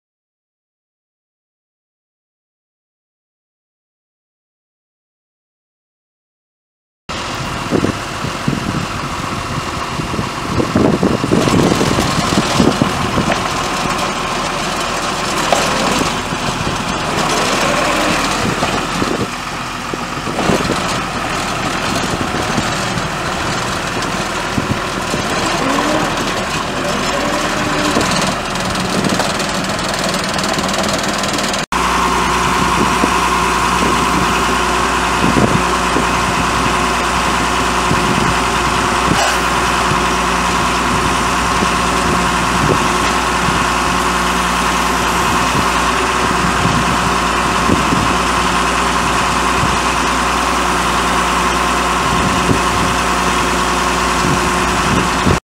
2000 American Augers DD-4 directional drill for sale | no-reserve Internet auction April 5, 2018
View a detailed listing of this item and place bids

2000 American Augers DD-4 directional drill
2,448 hours on meter
John Deere 6068TF155 six cylinder turbo diesel engine
155 HP
20'L x 7'W overall
(24) 10'L x 2 5/8" diameter drill stems
Auto rod loader
Hydraulic pump vise
FMC Bean Triplex pump
100 gpm
33,500 lbs thrust/pullback
4,000 lbs torque
Rear hydraulic stabilizers
12"W rubber tracks
Serial DD40570900
It's located in Sulphur, OK, and we're selling it to the highest bidder regardless of price.